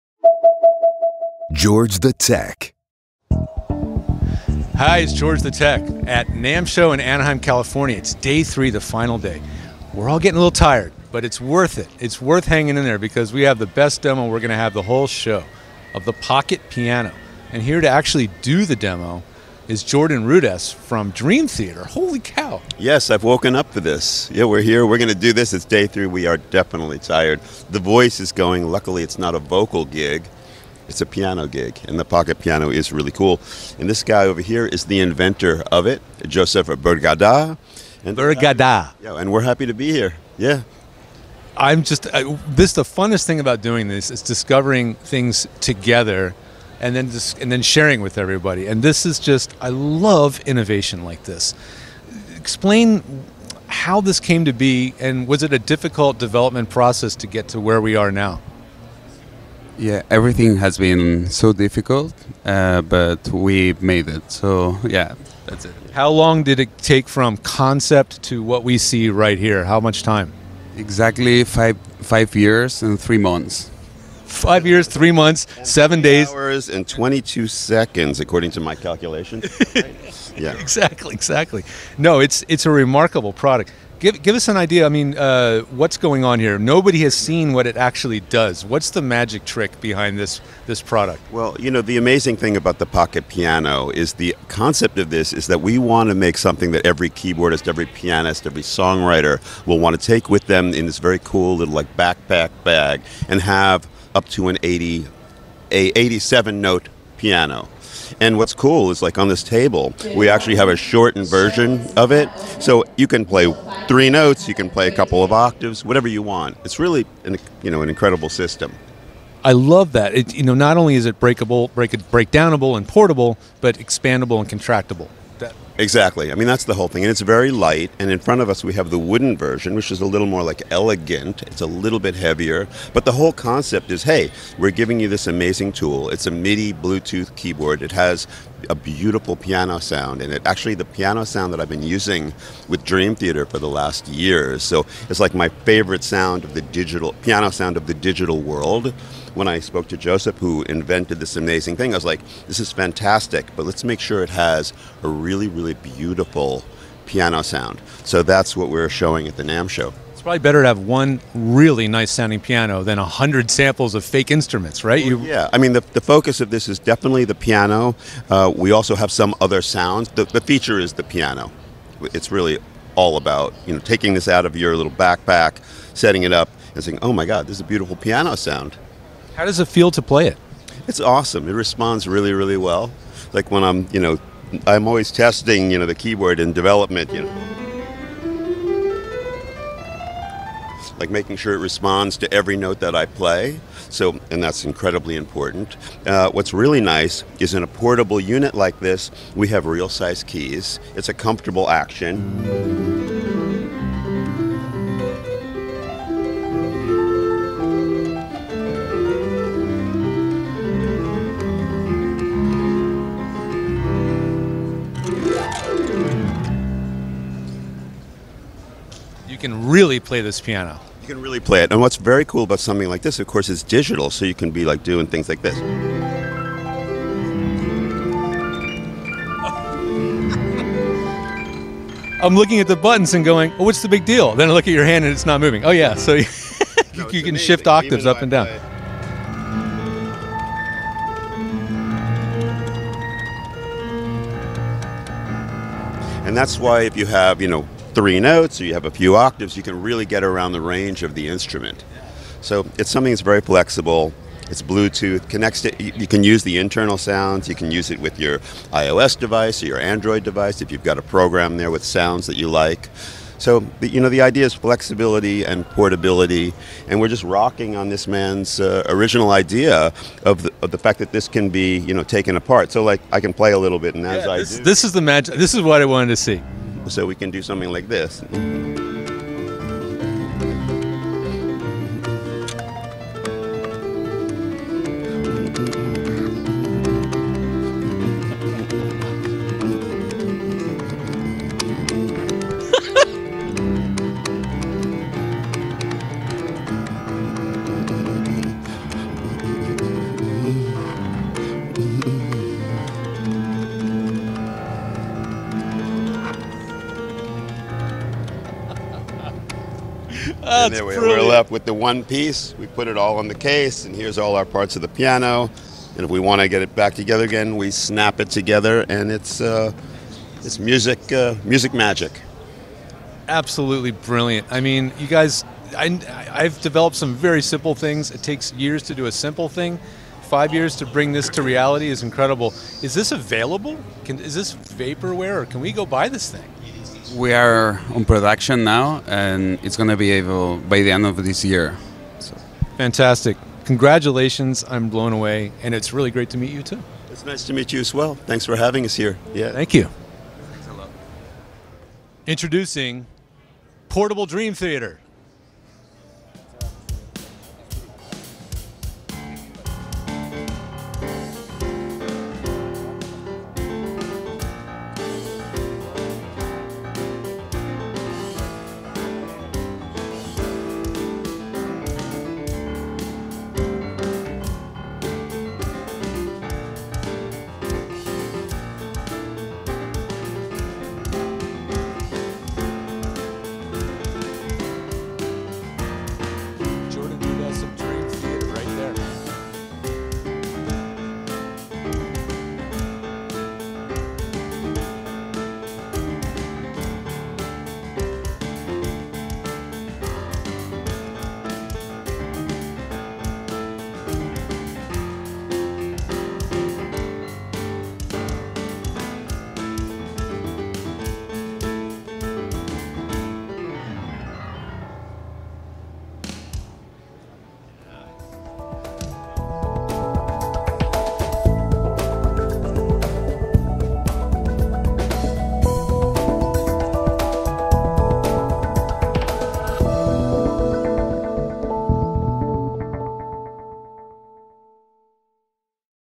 GTT NAMM 2022 Product Preview: Pocket Piano with Jordan Rudess, and a Freedrum Jam!
Josep Bergadà Bombí shows us his remarkable collapsable digital piano with Dream Theater's Jordan Rudess. A piano that expands and contracts to the size you need, with a beautiful piano sample, it is a piano first. It has bluetooth midi so you can control iOS and desktop computers for the ultimate portable performance piano and midi controller.
We were lucky to have Roy Moonen from Freedrum on hand to do a very impromptu and unrehearsed jam session with Jordan! Introducing: Portable Dream Theater :)
See the Office Hours Global interview with the founders of Freedrum:

Produced by Noah Sargent of Corporate Streams and officehours.global